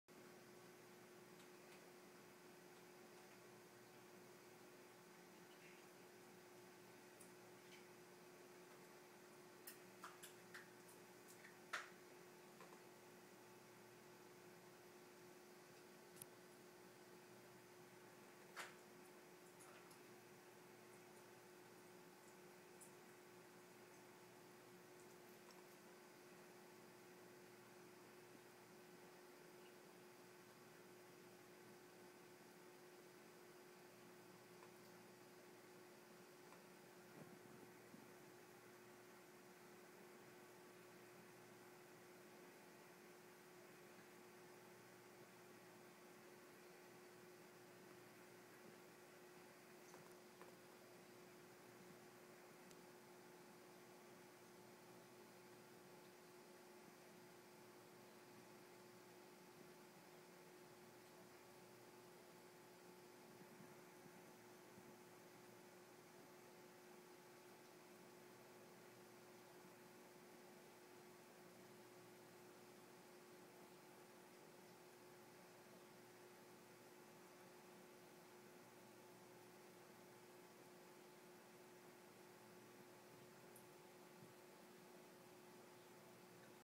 Niko caught on surveillance camera
Niko climbs 12 foot gate